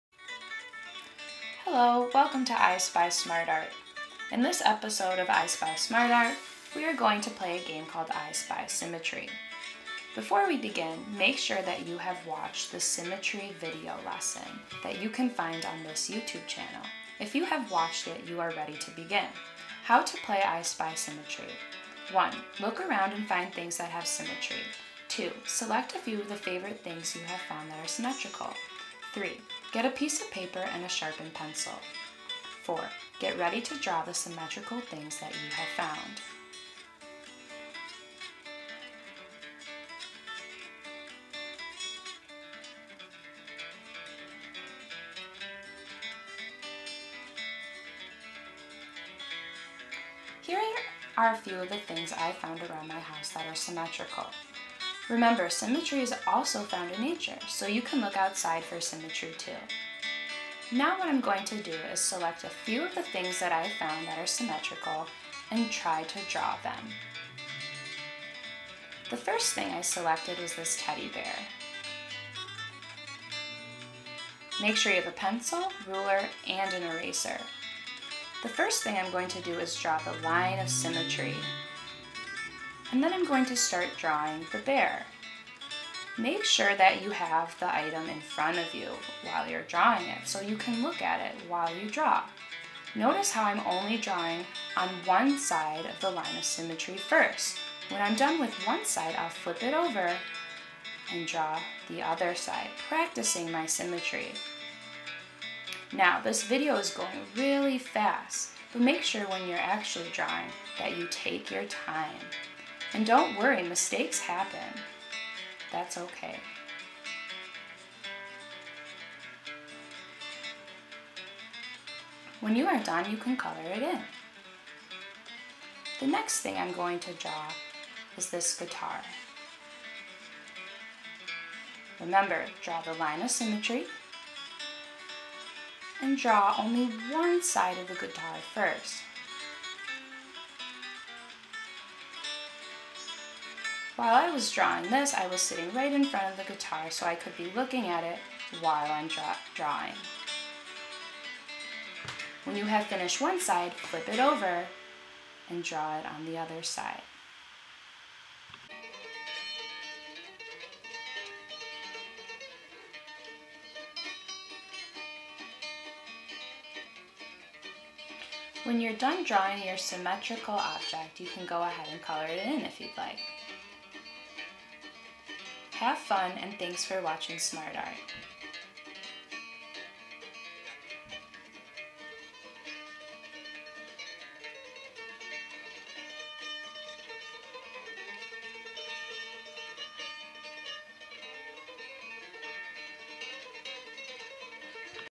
I Spy Symmetry
I Spy Symmetry is an art game that teaches students how to identify symmetry and how to practice drawing symmetrical objects.